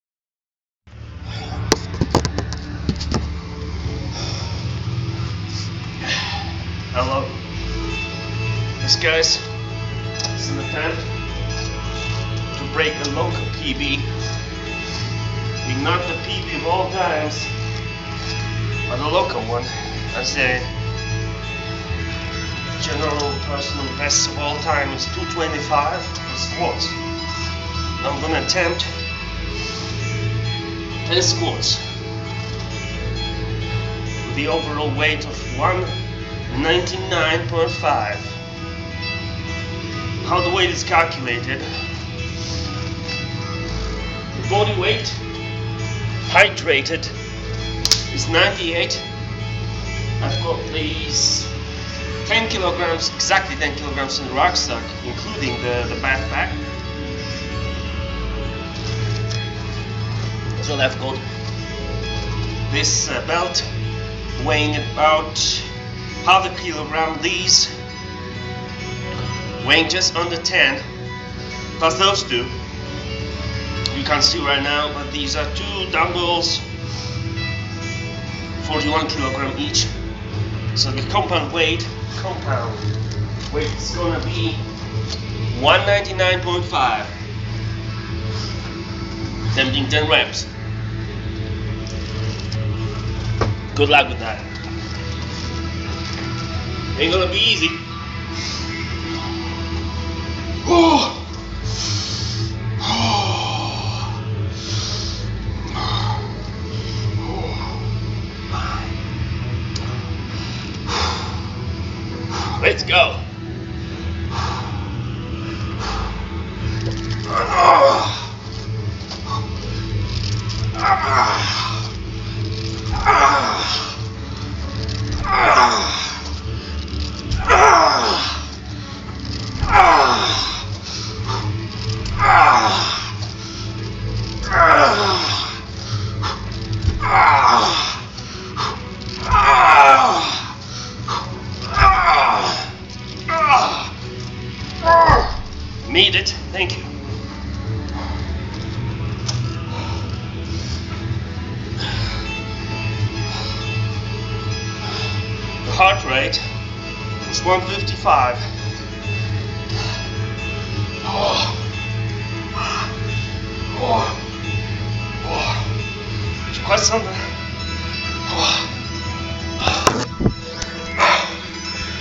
Local (after 40 yrs of age) PB in squats for reps, JungleCCat
This is my last months' PB in squats for reps, 10 squats @ overall 199.5 kg weight.

The weights were as follows:

Body weight: 98
backpack: 10
belt + weights at the belt: 10
dumbbells: twice 41kg